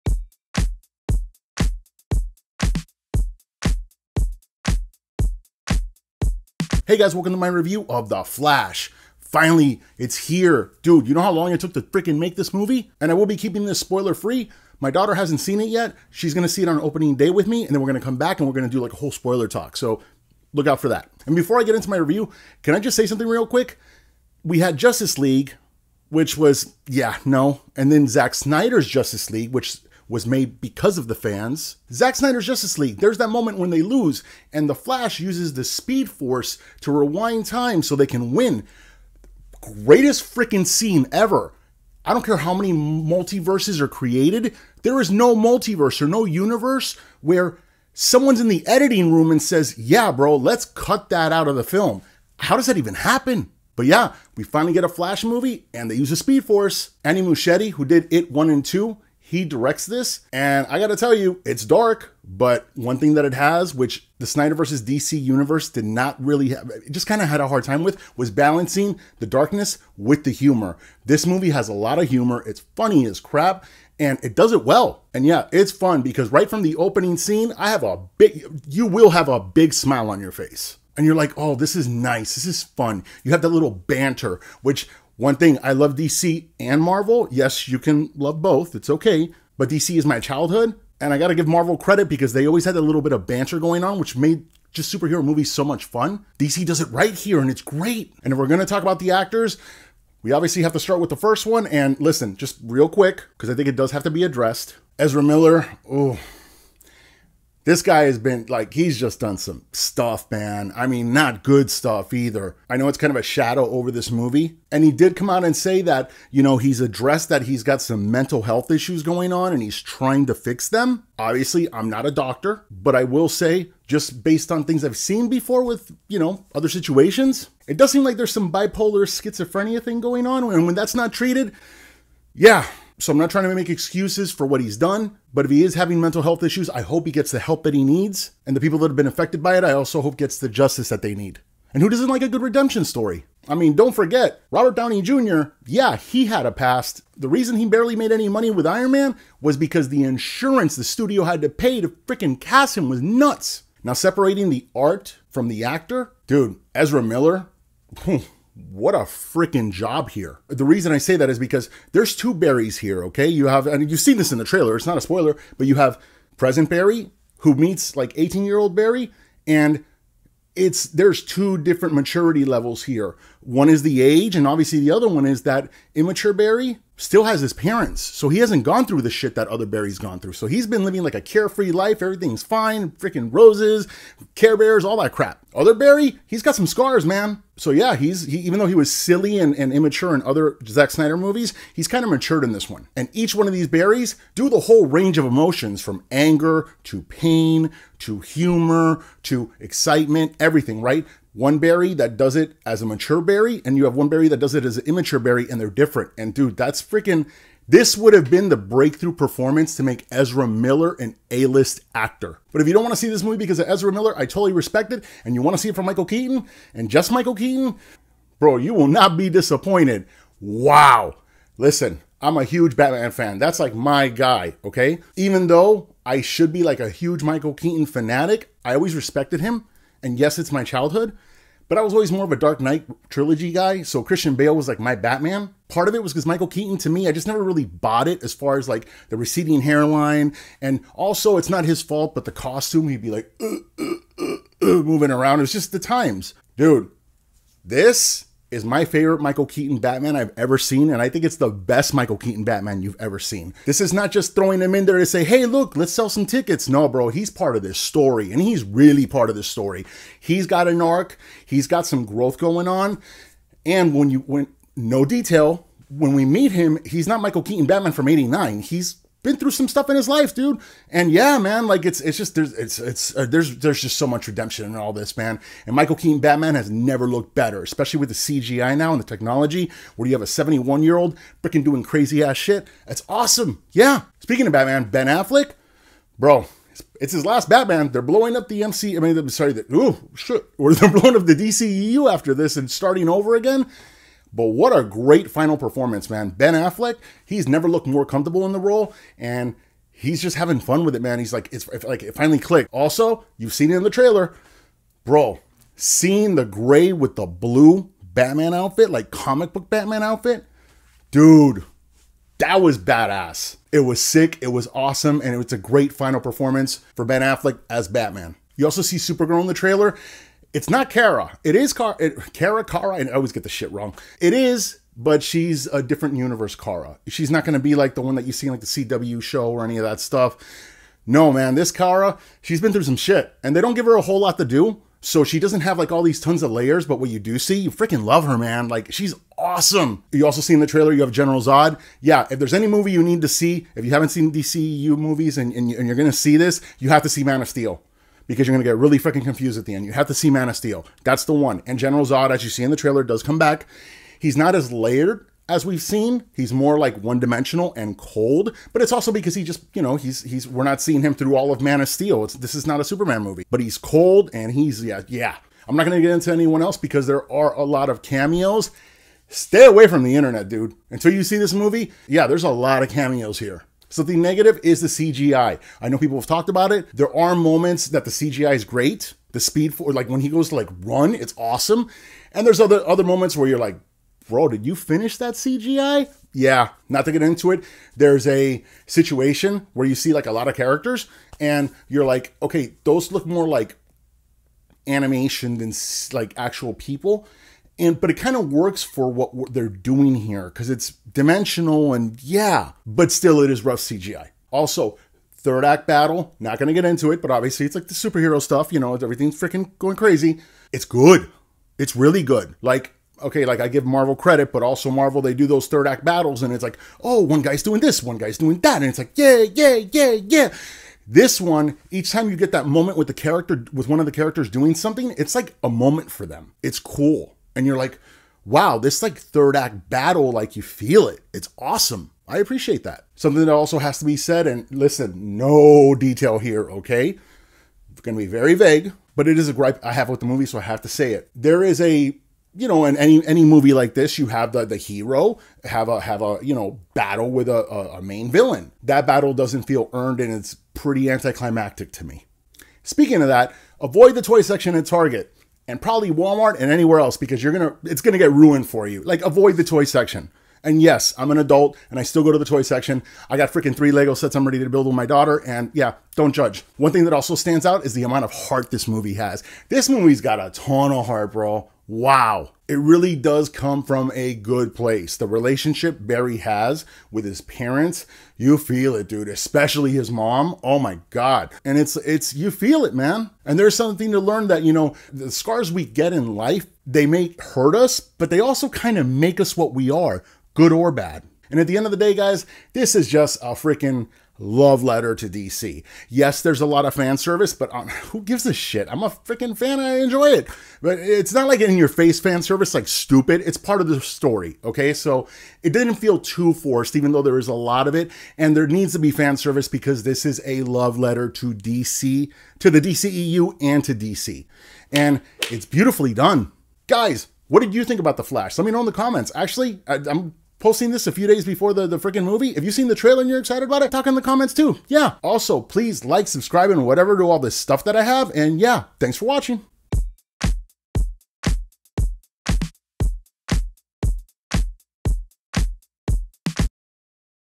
The Flash Review - No Spoilers
The Flash is almost here and I have to say there are some things in this movie you would have never thought you'd see in a film. Welcome to my The Flash Review. I will be keeping this spoiler-free, only revealing things that you would already know if you saw the trailers. If you are going to look up more content on the film, I would stick to the No Spoiler videos because there are a few things you want to avoid learning before your first viewing experience.

What did you think of the trailers and which was your favorite?